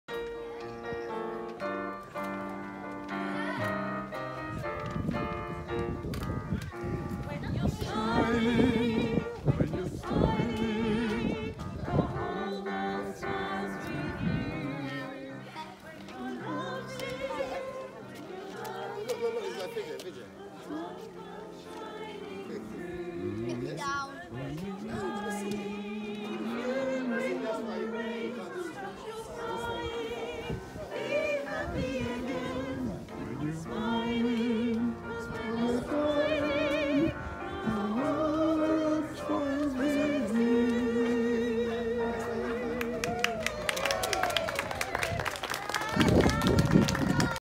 street singalong in Lockdown
Eleanor Meynell leads a street singalong for her neighbours in summer lockdown 2020